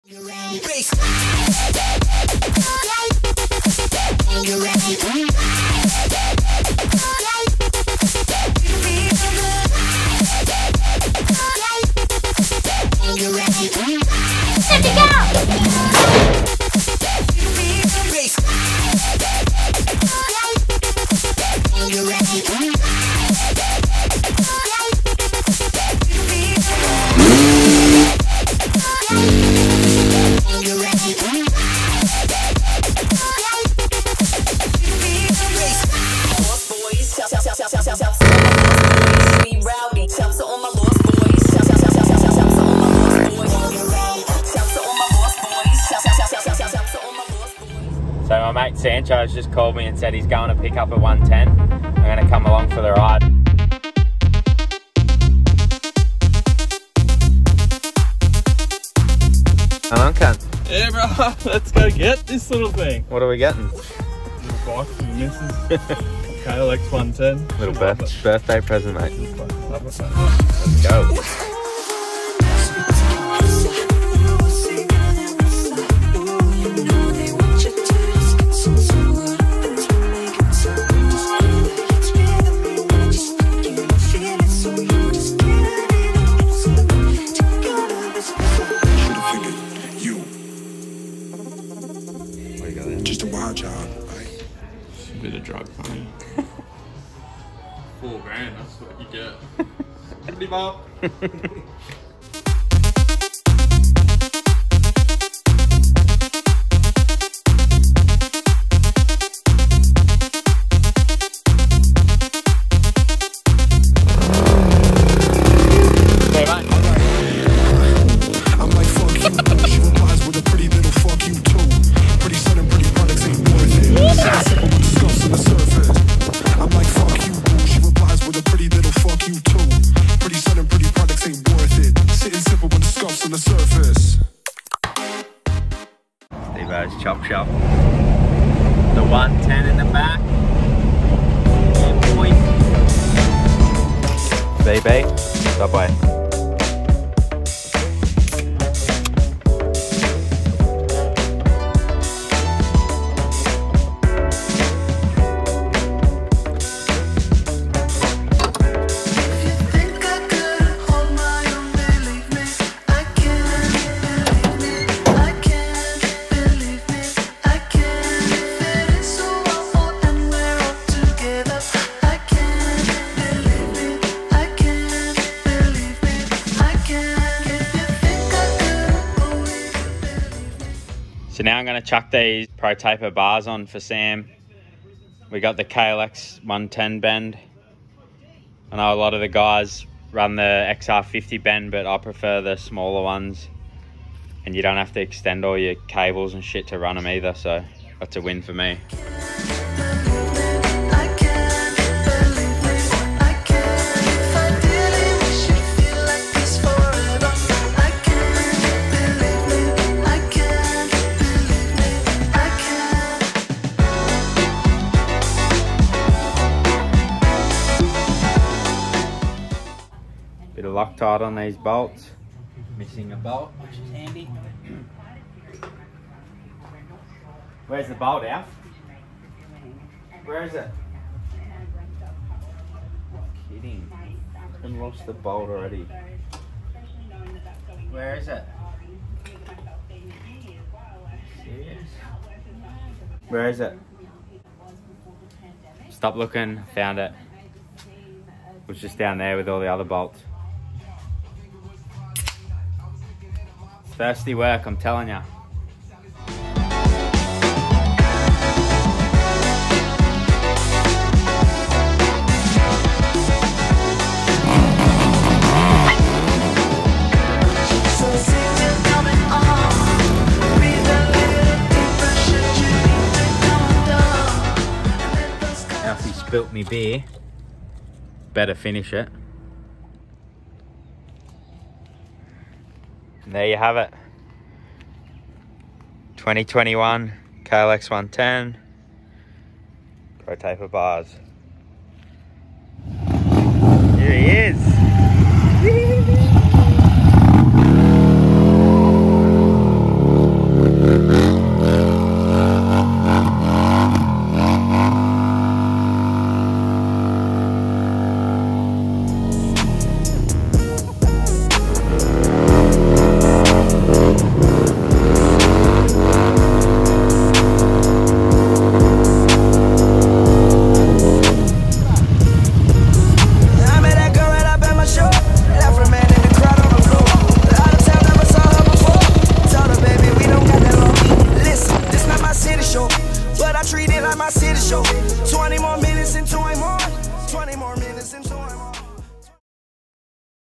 Friday arvo 110s, Harley skids and Beers!
@stevos.chop.shop and Sancho head to the dealership and pick up a brand new KLX110L, drink some beers, throw on some new bars then go for a few skids on Harleys. Enjoy!